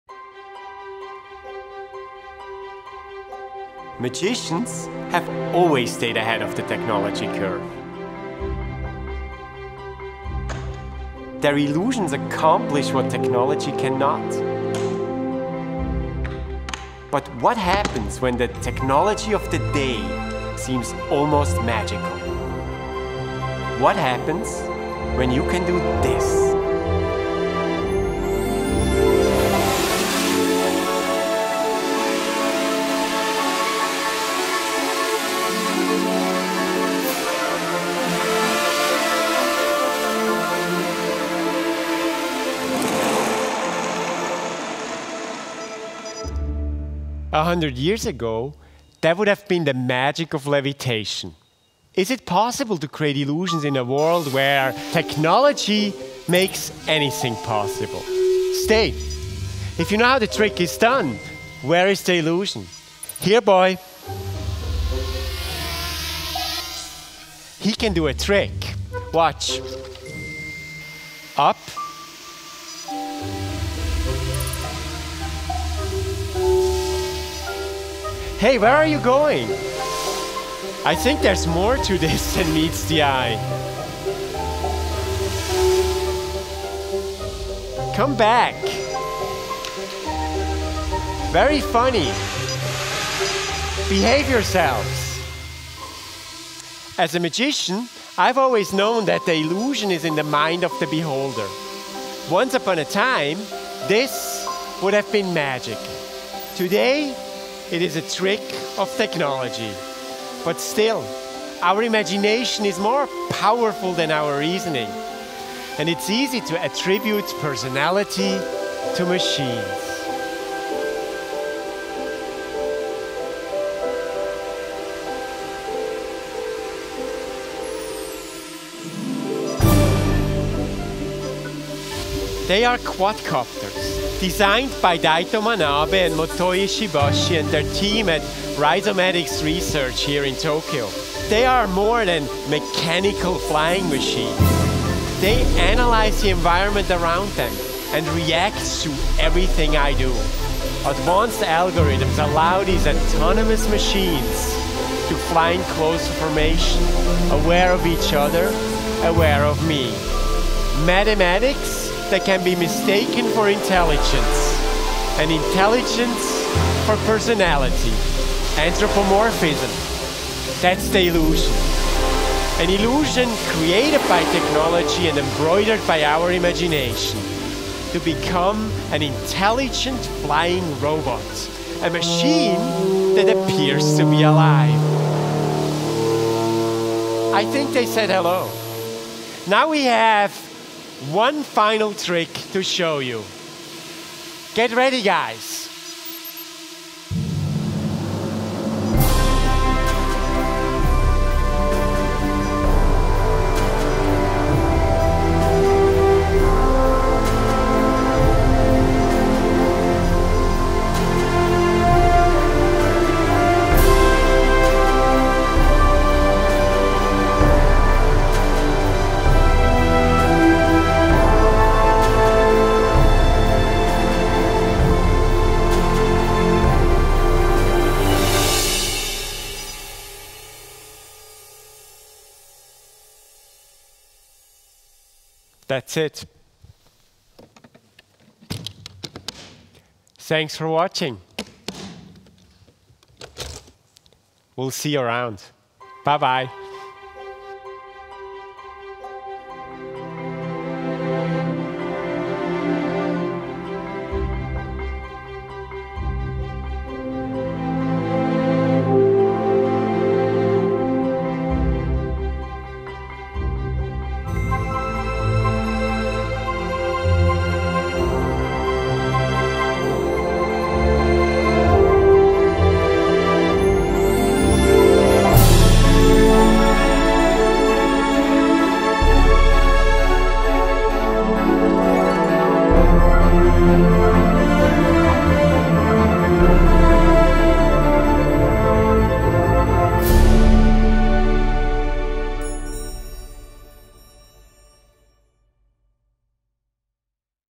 MagicLab - 24 Drone Flight
My latest production, utilizing autonomous drones, quadcopters, flying close to and around my body like a flock of trained birds. As legislation about the use and safety of flying drones is being debated around the world, this is a timely demonstration.

Working with Daito Manabe, Motoi Ishibashi and their team at Rhizomatiks Research in Tokyo, the goal is to create an intimate and artistic interaction between man and machine.